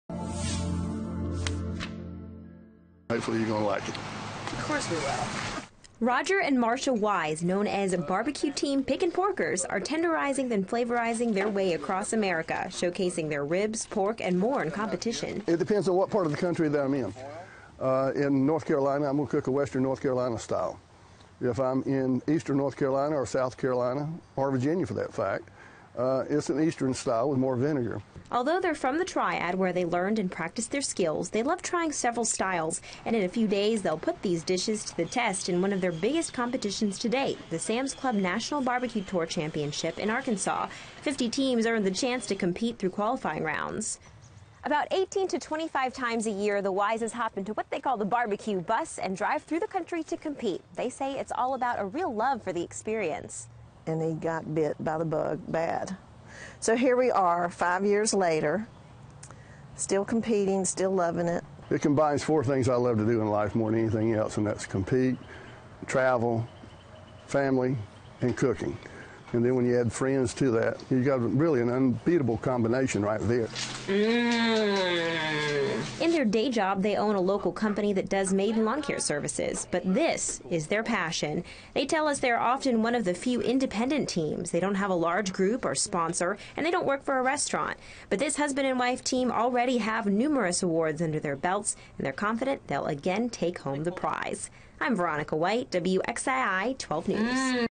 Pickin' Porkers ready for BBQ battle
A Triad couple is primed for battle in a major barbecue competition. WXII's Veronica White has the story.